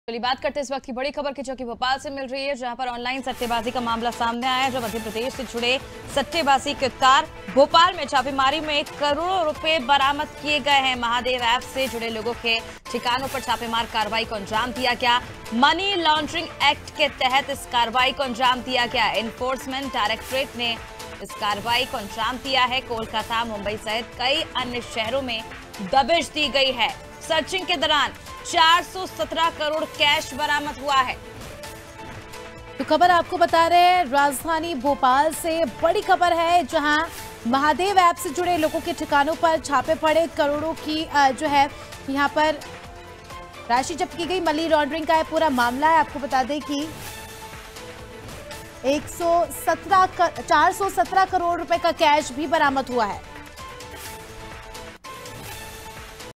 Bhopal :ऑनलाइन सट्टे बाजी का मामला, मध्य प्रदेश से जुड़े सट्टे बाजी के तार || Anaadi Tv
भोपाल:ऑनलाइन सट्टे बाजी का मामला
मध्य प्रदेश से जुड़े सट्टे बाजी के तार
भोपाल में छापेमारी में करोड़ों रुपए बरामद
महादेव ऐप से जुड़े लोगों के ठिकानों पर छापे
मनी लांड्रिंग एक्ट के तहत की कार्रवाई
एनफोर्समेंट डायरेक्टरेट ने की कार्रवाई
कोलकाता-मुंबई सहित कई अन्य शहरों में दबिश
सर्चिंग के दौरान 417 करोड़ कैश हुआ बरामद

COMING SOON
 DEN, AIRTEL